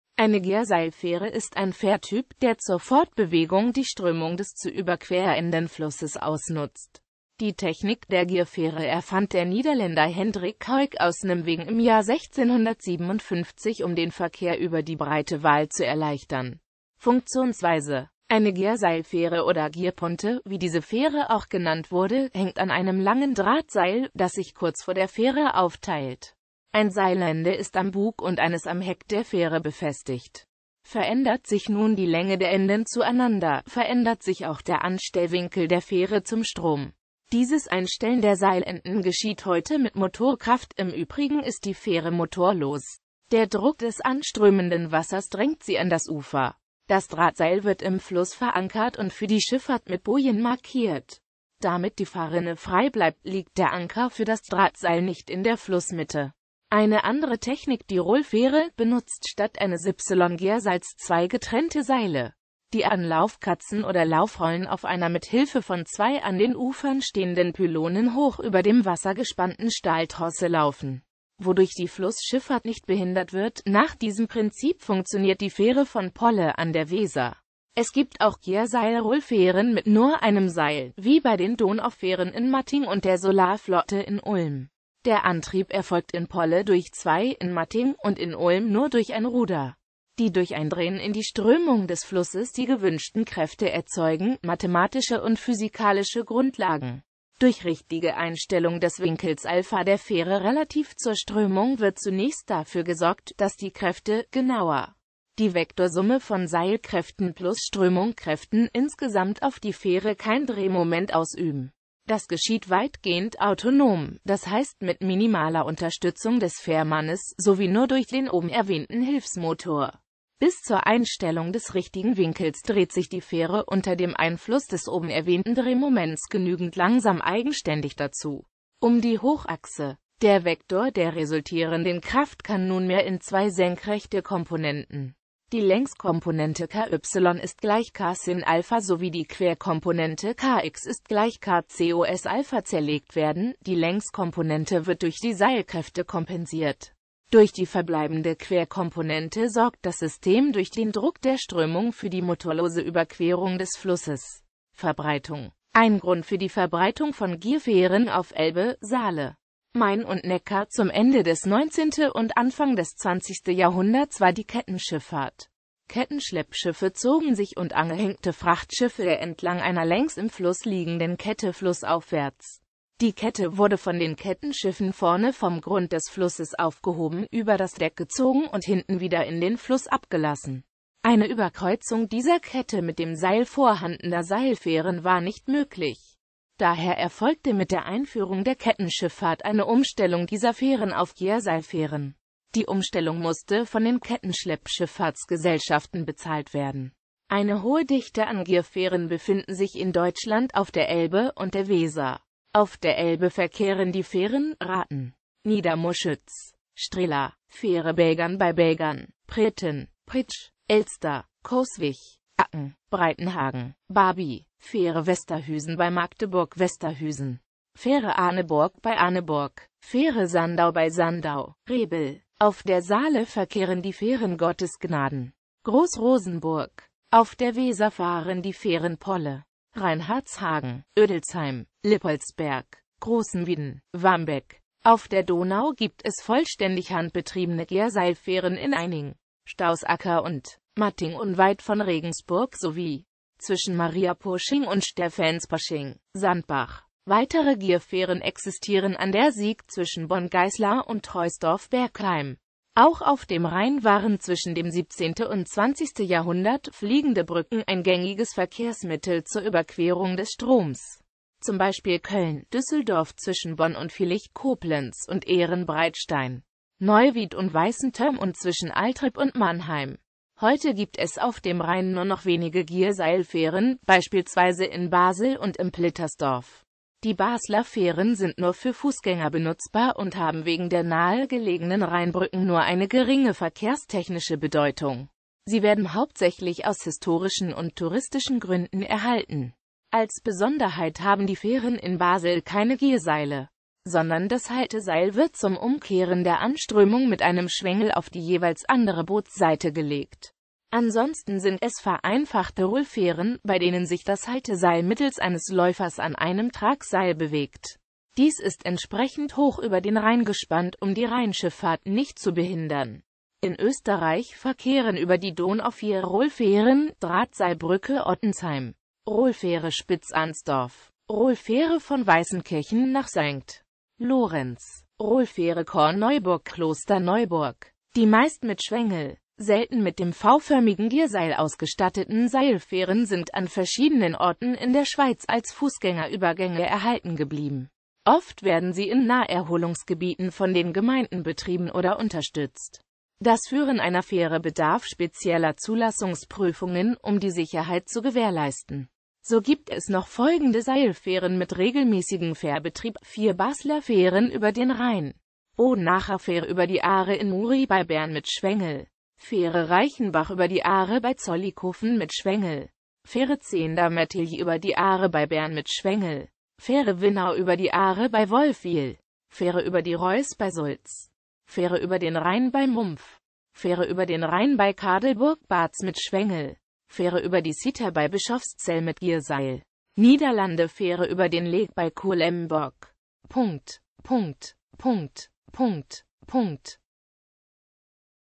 Gierseilfähre
Vielen Dank für Ihre Unterstützung:

Eine Gierseilfähre ist ein Fährtyp, der zur Fortbewegung die Strömung des zu überquerenden Flusses ausnutzt.Die Technik der Gierfähre erfand der Niederländer Hendrick Heuck aus Nimwegen im Jahr 1657, um den Verkehr über die breite Waal zu erleichtern.

Bild-Copyright-Informationen
Urheber Info: Seebeer in der Wikipedia auf Deutsch (Originaltext: Seebeer)